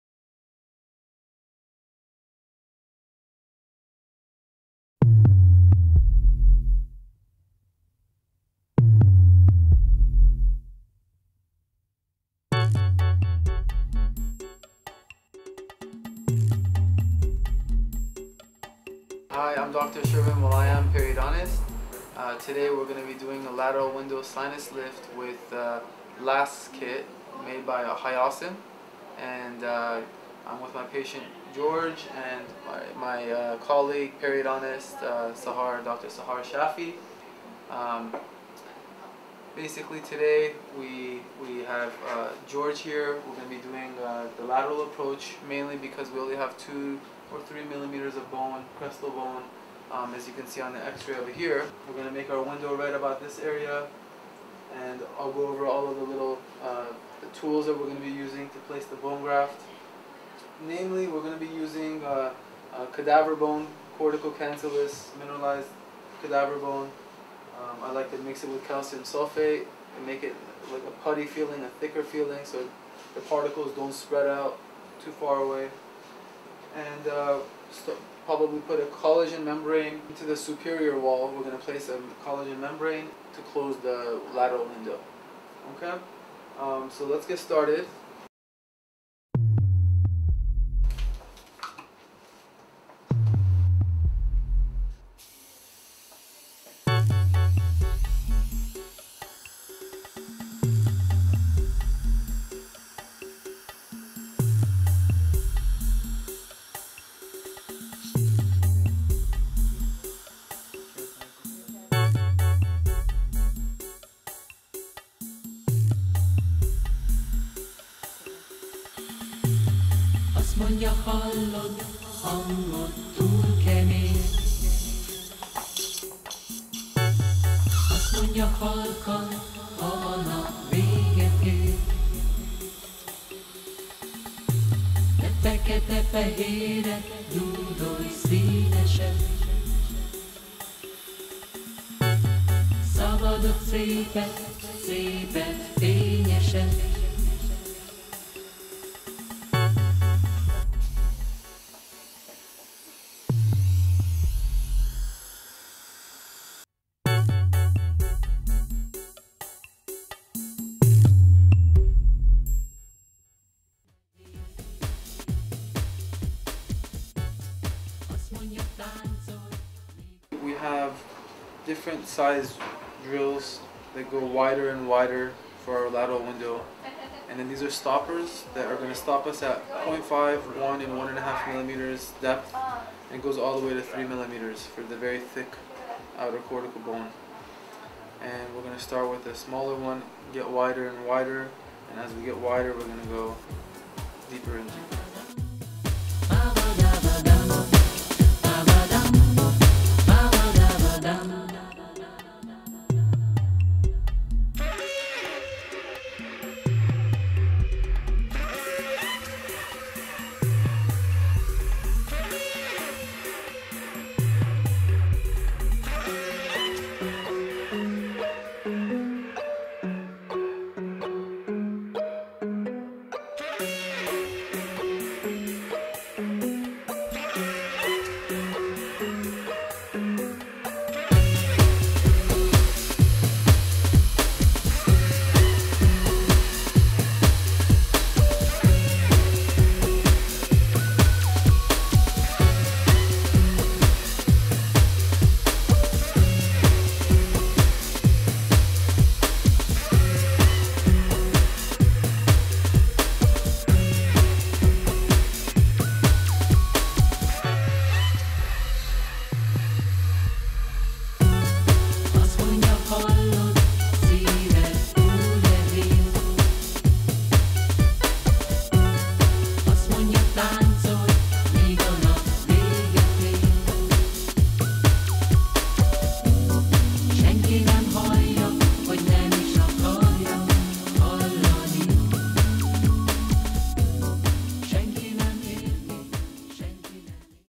Lateral Sinus Lift Surgery Part 1
Dr Shervin Molayem Periodontist performs the Lateral Sinus Lift
using Hiossen Las Kit.